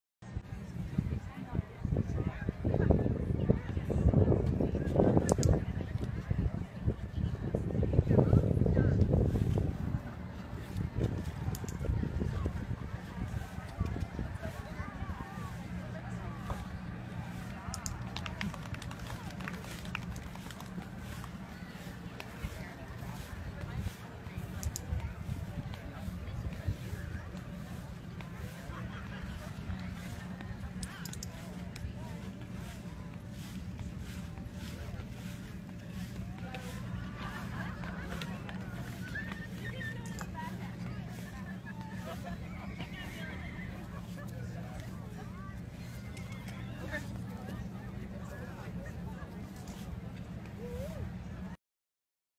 Llama Off-Lead Obstacle Course
Harlley completing an obstalce course off-lead at the Saanich Fair.